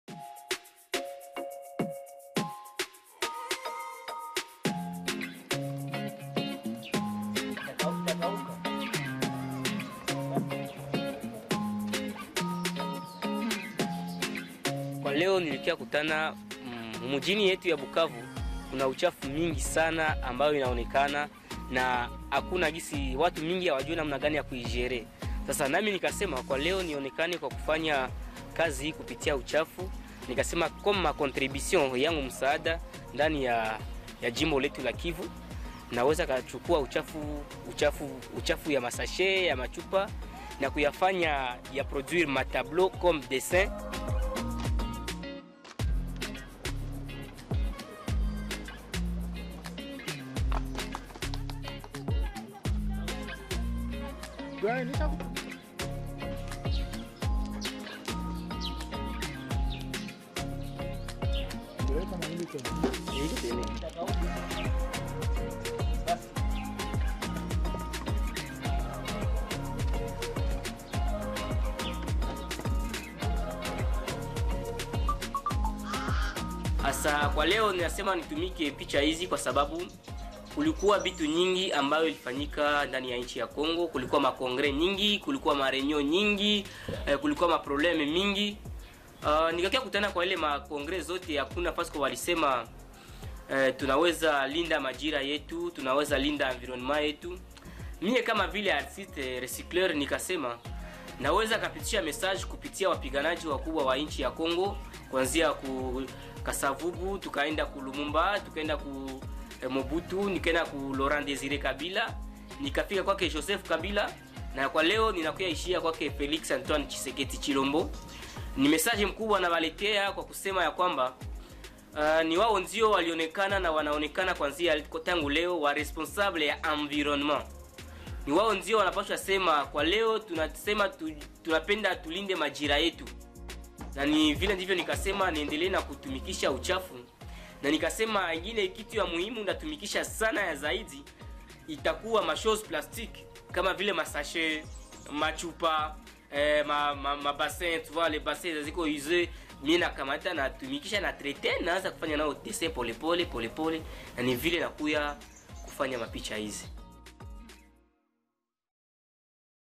Meet the artist who names and shames politicians with his recycled plastic portraits
Congolese artist Patrick Cikuru Cirimwami is using plastic waste to create shame portraits of politicians ‘responsible for the destruction of the environment’.